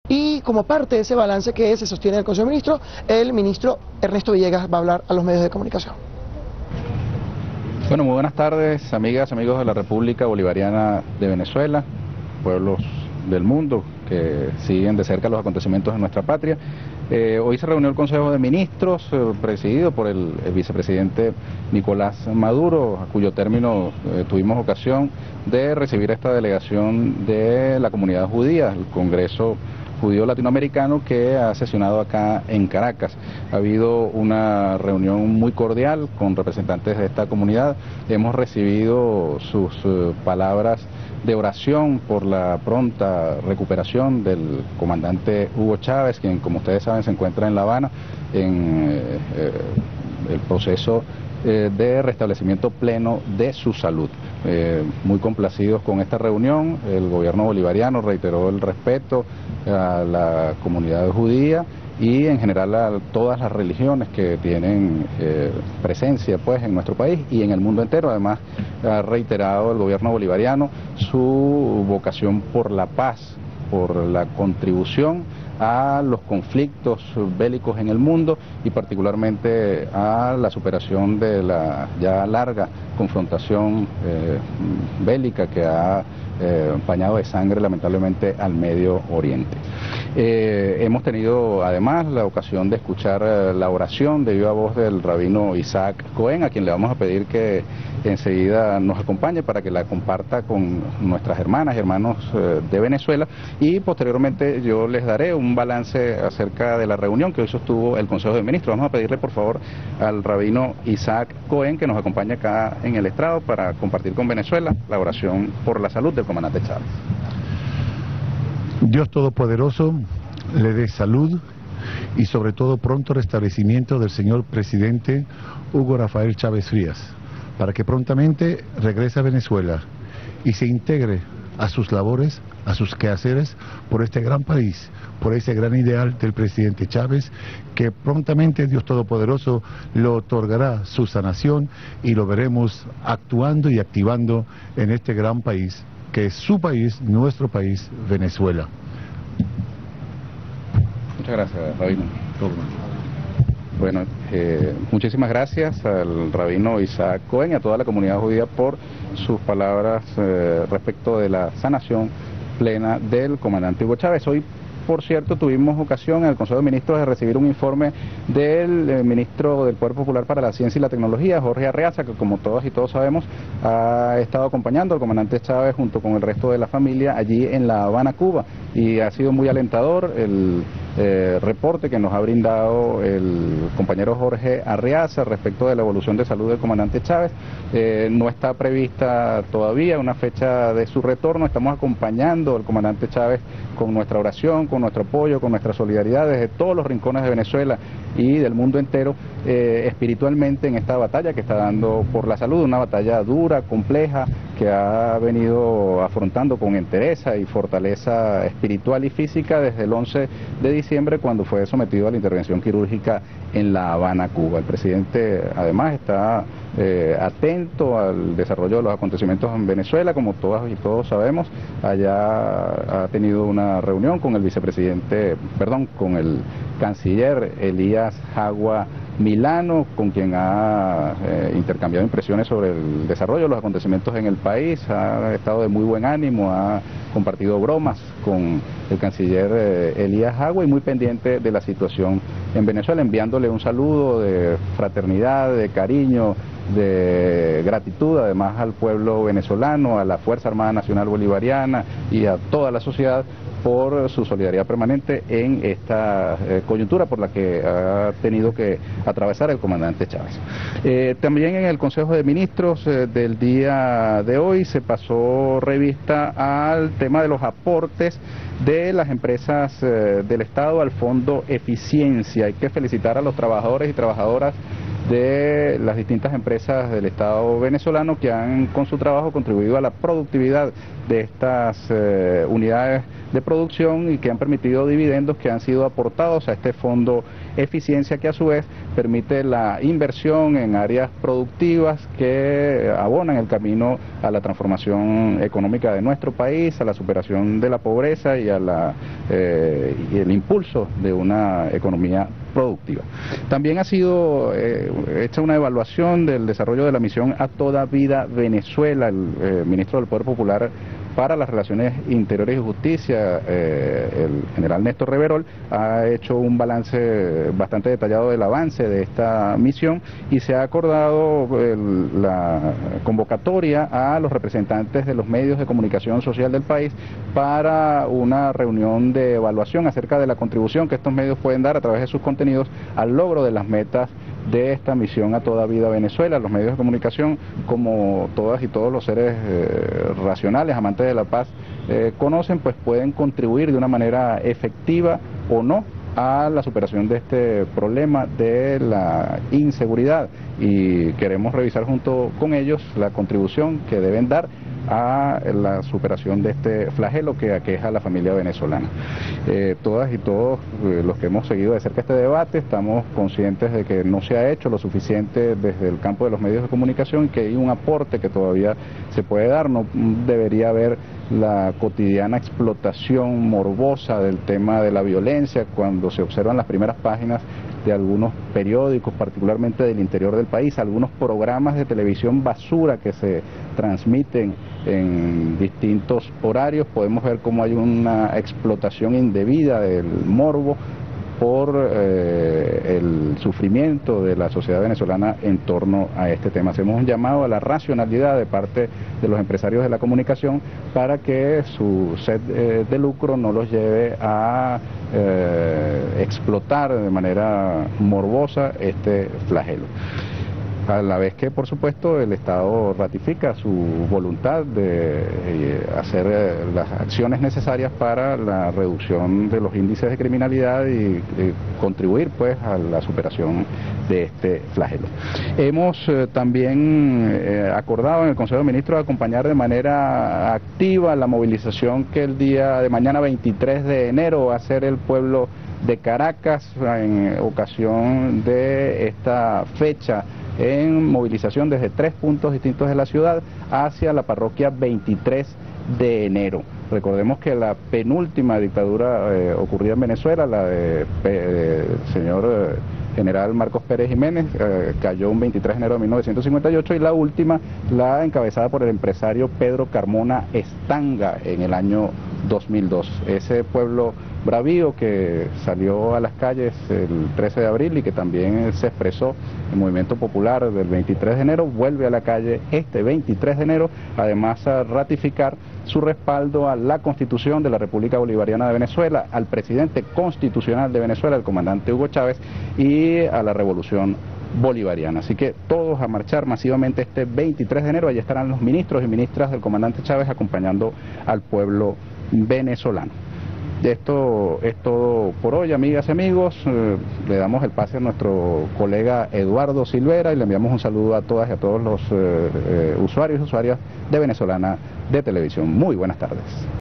Ernesto Villegas hace rueda de prensa al salir de Consejo de Ministros, 22 enero 2012
En la rueda de prensa, el rabino Isaac Cohén hace una oración por la pronta recuperación del Presidente Chávez. Informa del estado actual del Presidente Chávez y del mejoramiento de su salud, y hace un resumen del Consejo de Ministros realizado ese día.